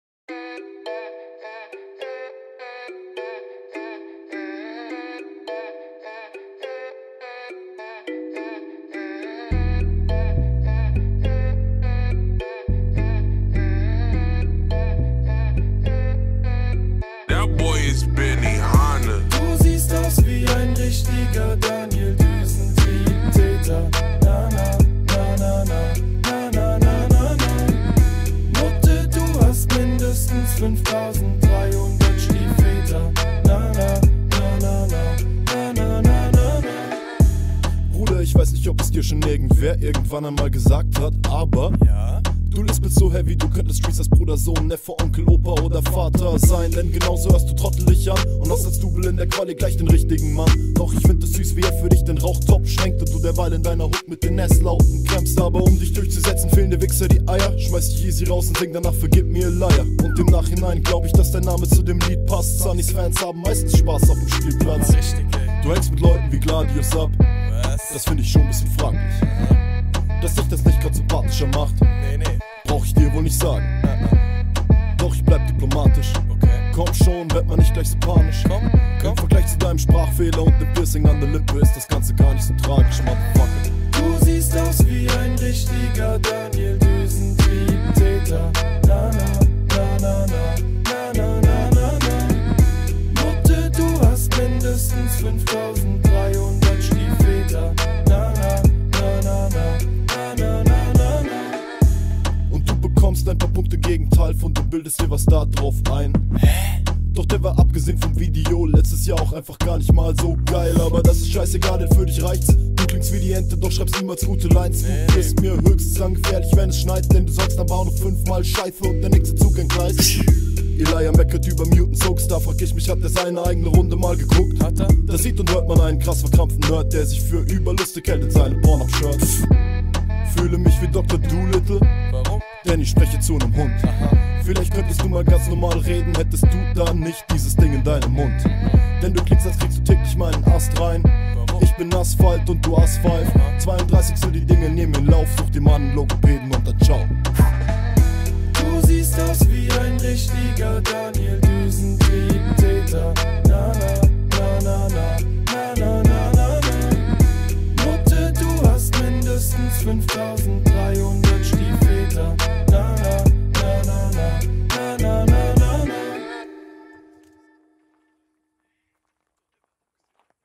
Egon McFly (ft. Gerd McFly) vs. Eliya | 32stel-Finale (05/32) - TNM Rap Battle S3
TNM Rap Battle Turnier S3 | Egon McFly (ft. Gerd McFly) vs. Eliya | 32stel-Finale (05/32)

Beschreibung :

Musik
Text: Egon Mcfly
Mix/Master: Gerd Mcfly
Beat: Benihana Boy
Hook: Gerd Mcfly

Kamera: Gerd Mcfly
Schnitt und Bearbeitung: Gerd Mcfly

mcfly_family

Hook:
Du siehst aus wie ein richtiger Daniel Düsentriebtäter nana na na na na na nana na /
Nutte du hast mindestestens 5300 Stiefväter nana na na na na na nana na/

Bruder ich weiss nicht ob es dir schon irgendwer irgendwann einmal gesagt hat aber/
Du lispelst so heavy du könntest Recers Bruder Sohn Neffe Onkel Opa oder Vater sein
Denn genauso hörst du Trottel dich an /
Und hast als Double in der Quali gleich den richtigen Mann/
Doch ich find es süß wie er für dich den Rauchtopf schwenkt/
Und du derweil in deiner Hook mit den s Lauten kämpfst/
Aber um dich durchzusetzen fehlen dir wichser die Eier/
schmeiß dich easy raus und sing danach vergib mir eliyah/
Und im Nachhinein glaub ich dass dein Name zu dem Lied passt/
Sunnys Fans haben meistens Spaß aufm Spielplatz/
Du hängst mit Leuten wie Gladius/
Das find ich schon ziemlich fraglich/
Das dich das nicht grad symphatischer macht/
Brauch ich dir wohl zu sagen/
Doch ich bleib diplomatisch/
komm schon werd Mal nicht gleich so panisch/
Im Vergleich zu deinem Sprachfehler und dem dem Piercing an der Lippe ist das ganze garnicht so tragisch/
Hook

Und du bekommst ein paar Punkte gegen Talf und du bildest dir was darauf ein/
Doch der war abgesehen vom Video letztes Jahr auch einfach garnicht Mal so geil/
Aber das ist scheissegal denn für dich reicht's/
Du klingst wie die Ente doch schreibst niemals gute Lines/
Du wirst mir  höchstens dann gefährlich wenn es schneit/
Denn du sagst am Bahnhof 5 Mal scheisse und der nächste Zug entgleist/
Eliyah meckert über muutens hooks/
Da frag ich mich hat seine eigene Runde Mal geguckt?/
Da sieht und hört man einen krass verkrampften Nerd/
Der sich für überlustig hält in seinem Pornhub Shirt/
Fühle mich Dr.Doolitle/
Denn ich sprech hier zu nem Hund/
Vielleicht könntest du Mal ganz normal sprechen hättest du da nicht dieses Ding in deinem Mund/
Denn du klingst als kriegst täglich meinen Ast rein/
Ich bin Asphalt und du US5/
32tel die Dinge nehmen ihren Lauf/
Such dir mal nen Logopäden und dann Ciao/

Hook